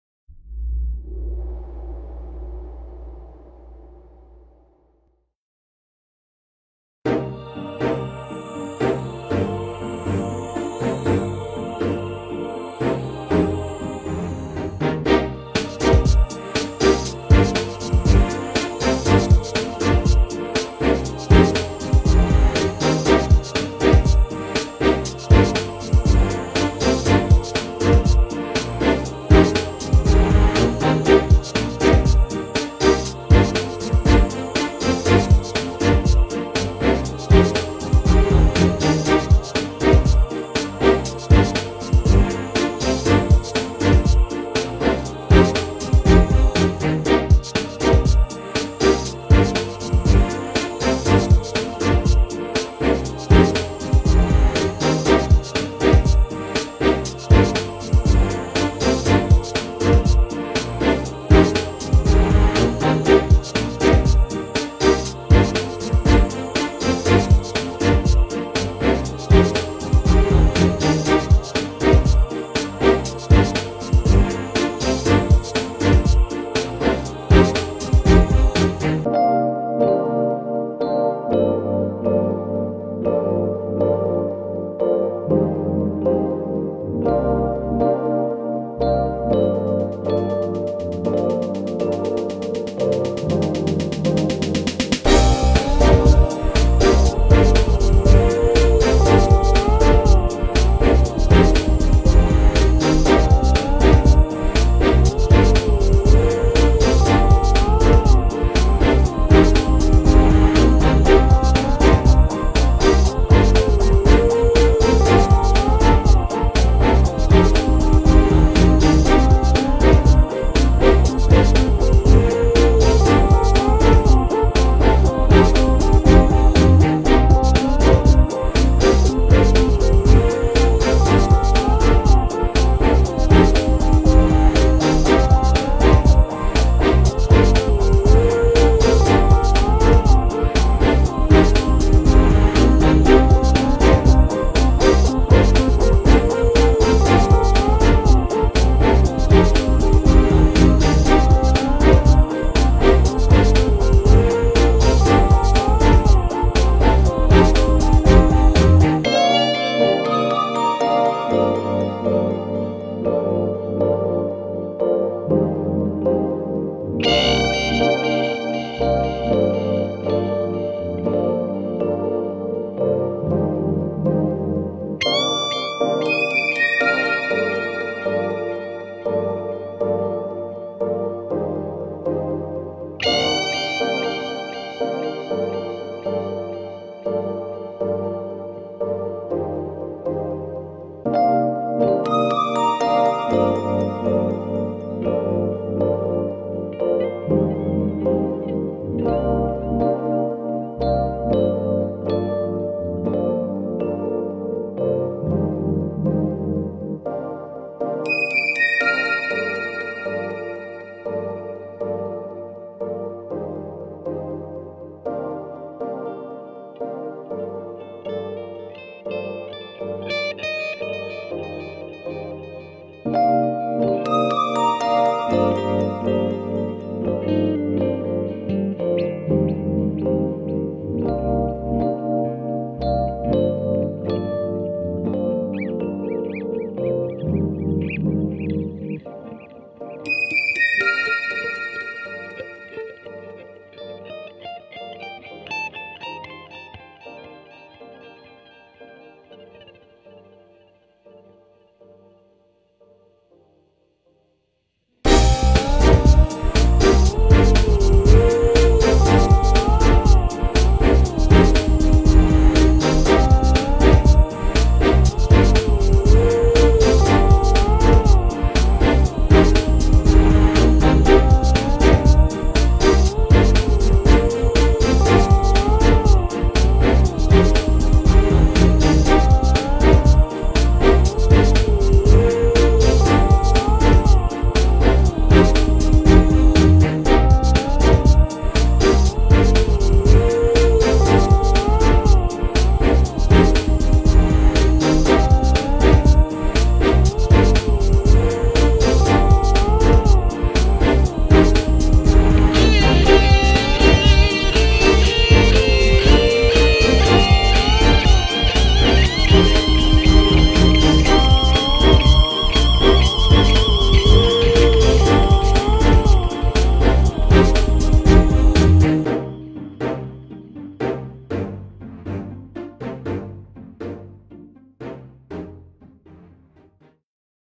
Bear in the Forsest (Time Lapse Digital Painting)
I got some great shots at the Los Angeles Museum of Natural History, and got inspired. Hope you enjoy the piece.